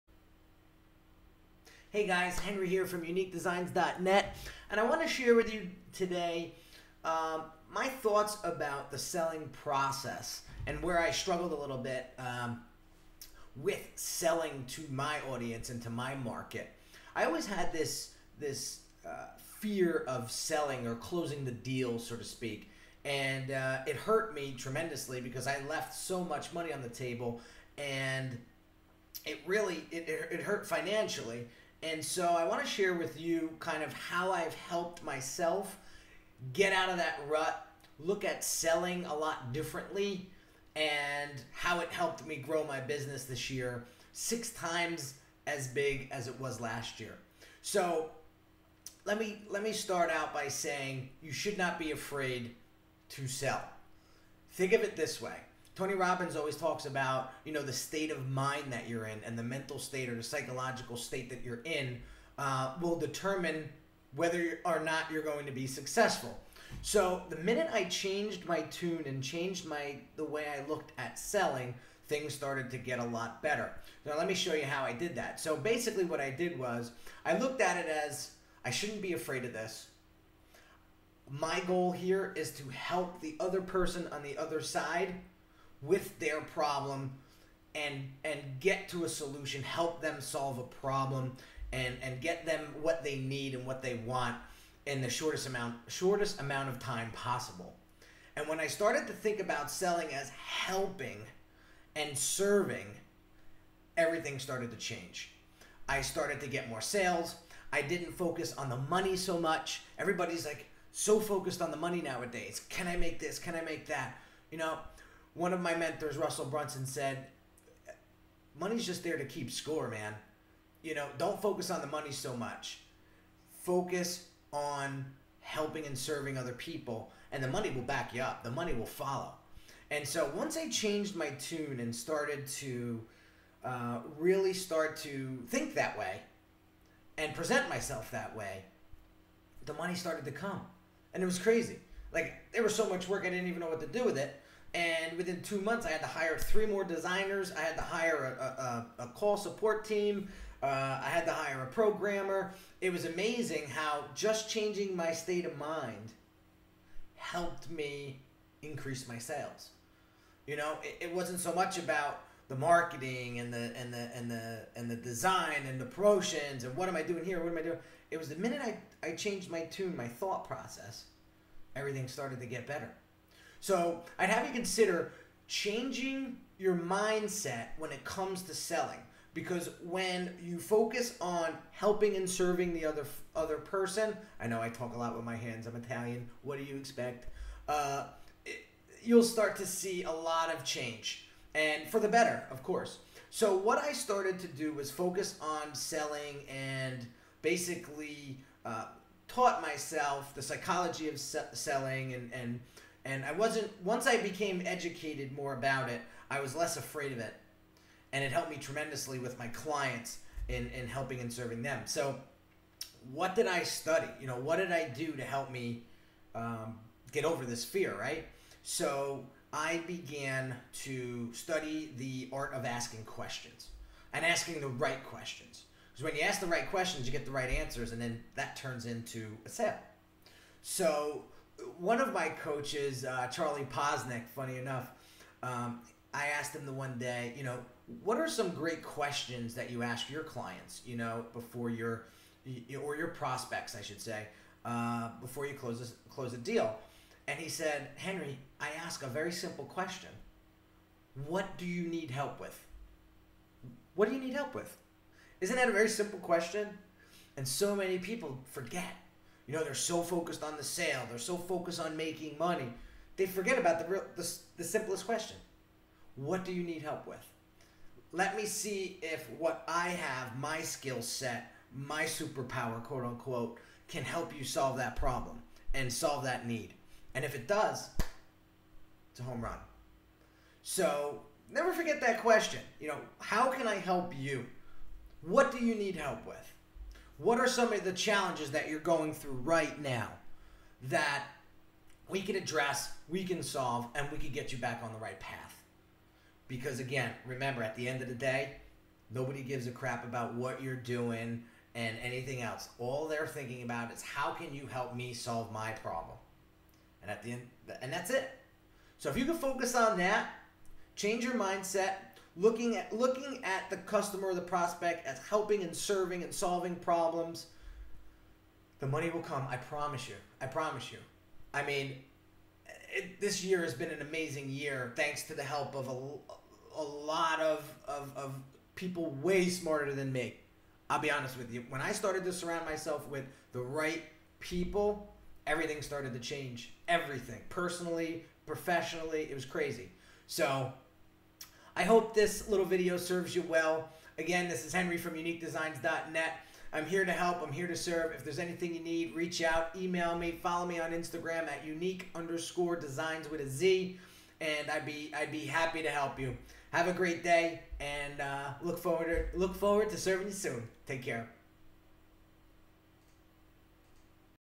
How Changing Your State Of Mind Can Help You Sell More
In this short video, I'll share with you how once I changed my state of mind about selling, helped me 6x my business in just 11 months!